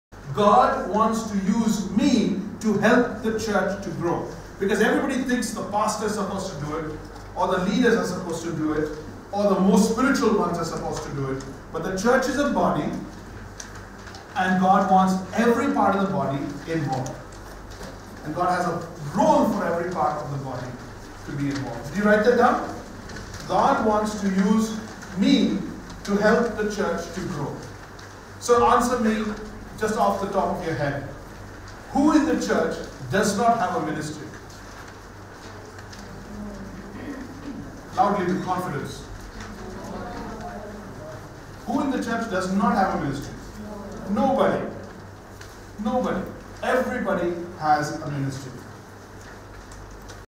God wants to use me to help the church grow
Everybody thinks the Pastor is supposed to do it or the leaders are supposed to do it or the more 'Spiritual ones' should be doing it. BUT the church is a body and God wants every part of the body involved.